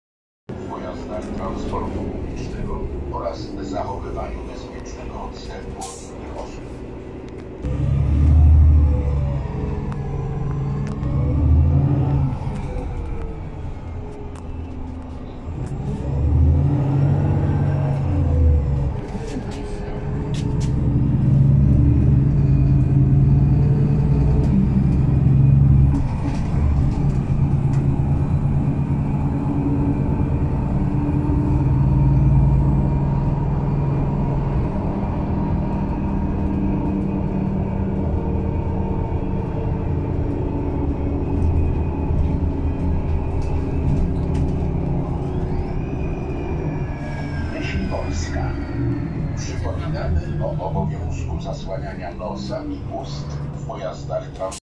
Przejazd solarisem urbino IV GENERACJI linia 186 w kierunku Myśliborska
Cześć dziś się przejedziemy solarisem urbino IV Generacji linia 186 w kierunku Myśliborska